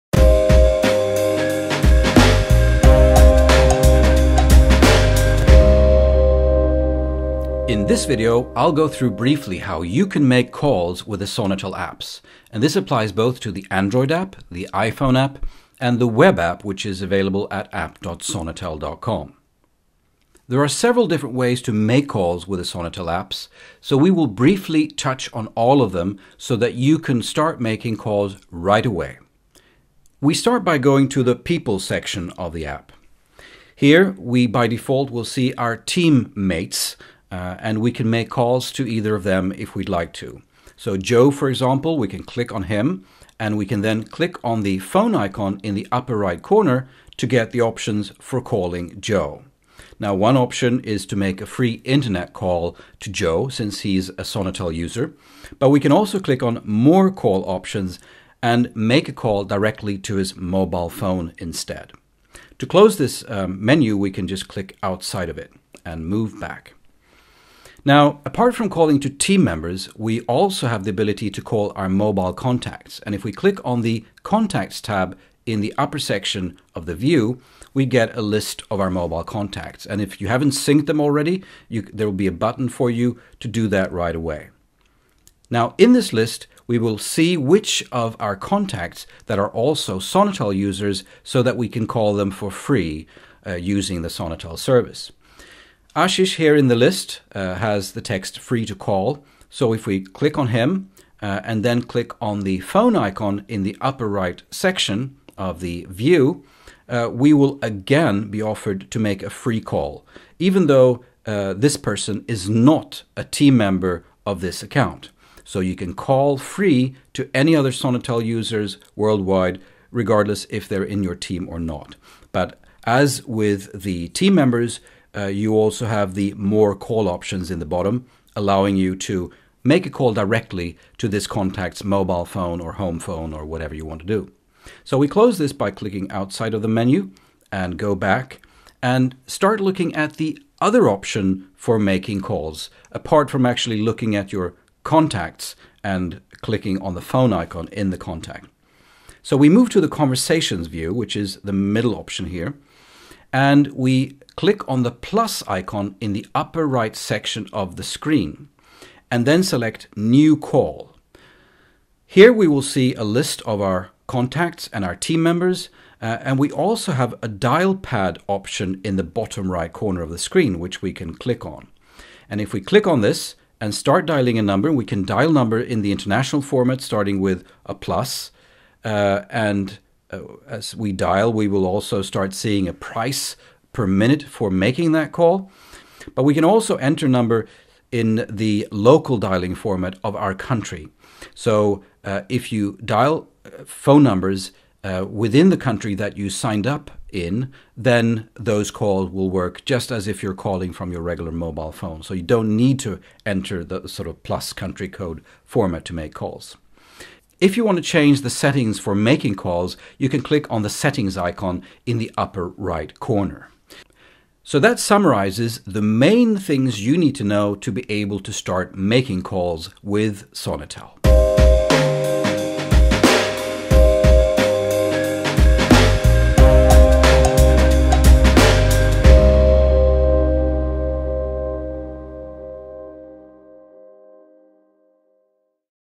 How to make calls with the Sonetel apps
Learn how to make free and low cost international calls with the free Sonetel apps.

This tutorial shows how to make calls in the new Sonetel apps.

There are also older legacy apps available from Sonetel, but those are not covered in this video.